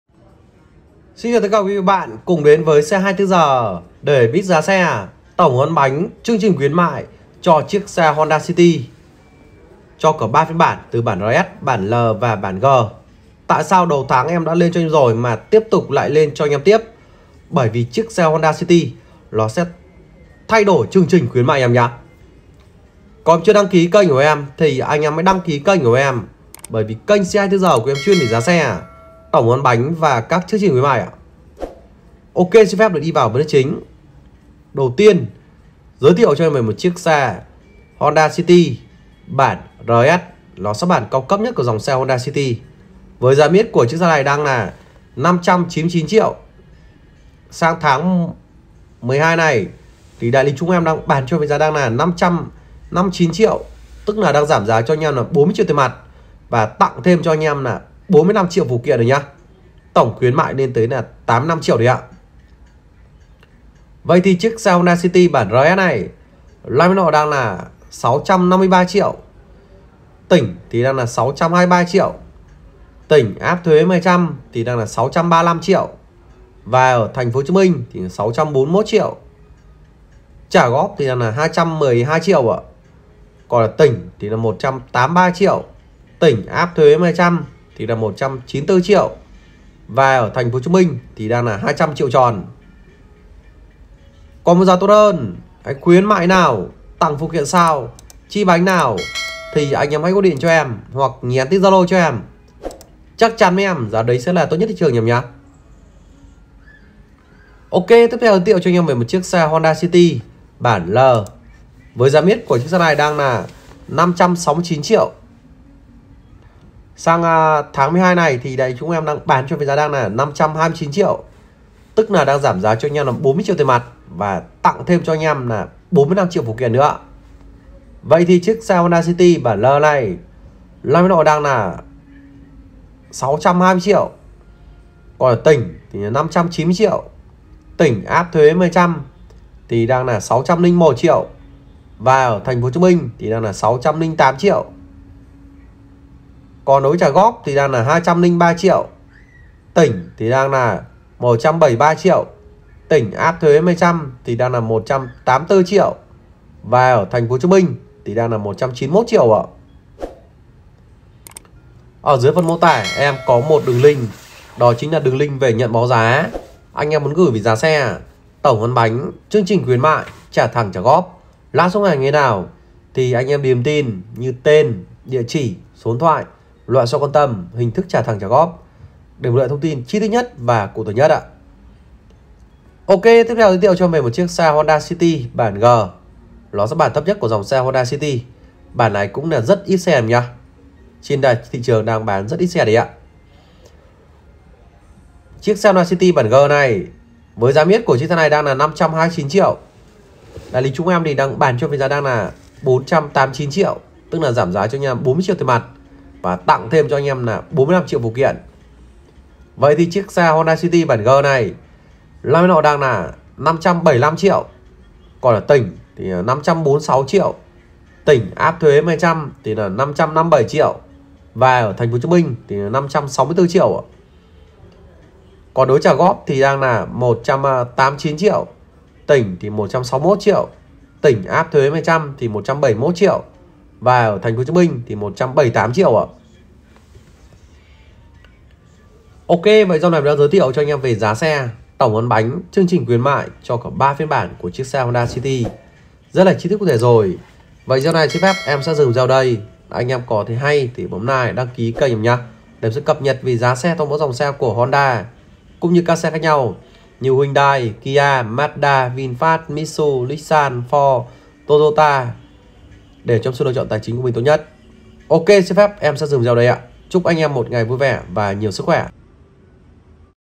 ✅ Giá xe Honda City 2022 Khuyến mại 85tr giữa tháng 12
*Giá xe Honda City 2022:
- Giá xe Honda City RS 2022
  + Tổng lăn bánh Hà Nội: 678tr
  + Tổng lăn bánh Tỉnh 10% thuế: 648tr
  + Tổng lăn bánh Tỉnh 12% thuế: 660tr
  + Tổng lăn bánh TP Hồ Chí Minh: 665tr
- Giá xe Honda City L 2022
  + Tổng lăn bánh Hà Nội: 645tr
  + Tổng lăn bánh Tỉnh 10% thuế: 615tr
  + Tổng lăn bánh Tỉnh 12% thuế: 626tr
  + Tổng lăn bánh TP Hồ Chí Minh: 633tr
- Giá xe Honda City G 2021
  + Tổng lăn bánh Hà Nội: 600tr
  + Tổng lăn bánh Tỉnh 10% thuế: 571tr
  + Tổng lăn bánh Tỉnh 12% thuế: 582tr
  + Tổng lăn bánh TP Hồ Chí Minh: 589tr

**Nếu AE muốn xem giá xe và các tính năng như thế nào thì hãy click vào đường link nhé.
* Honda:
*Hyundai:
* Toyota:
* Mitsubishi:
 - Mitsubishi Attrage 2022 Premium:
 - Mitsubishi Attrage 2022 MT: ...
* KIA:
* Các màu xe của Honda City 2022:
 - Màu đen (NKA)
 - Honda City 2022  Màu trắng Ngà.
 - Honda City 2022 Màu Ghi Bạc.
 - Honda City 2022 Màu Titan.
 - Honda City 2022 Đỏ.
 - Honda City 2022 Xanh Đậm.
- Honda City 2022 Đen.